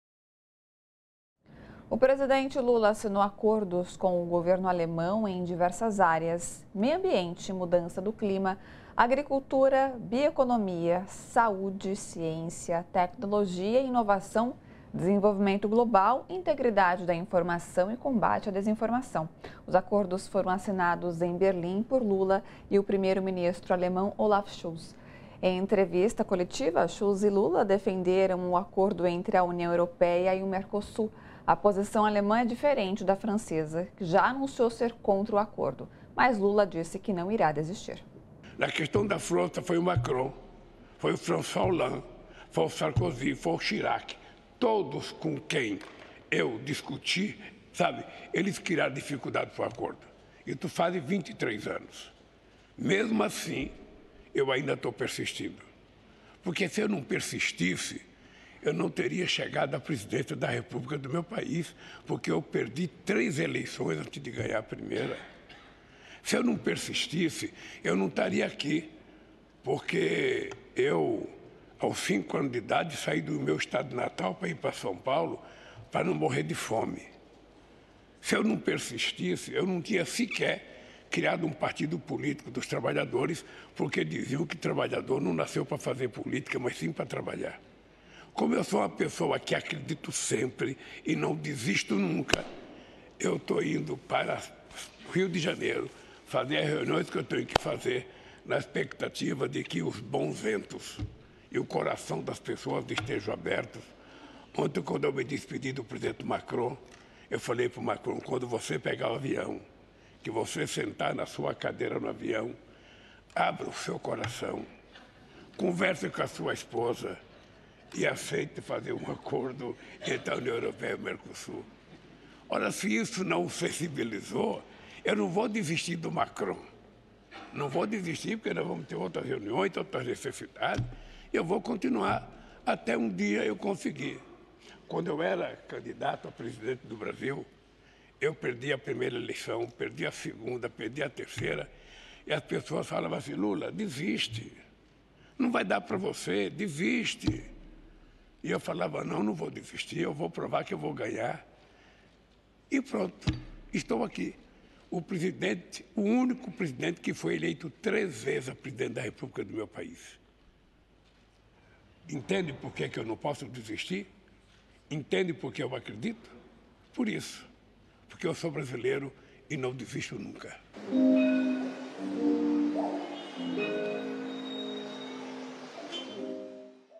Lula encontra primeiro-ministro da Alemanha e líderes defendem acordo entra Mercosul e UE
A posição alemã é diferente da francesa, que já anunciou ser contra o acordo. Lula e Olaf Scholz, também assinaram acordos em diversas áreas entre elas ciência, tecnologia e meio ambiente.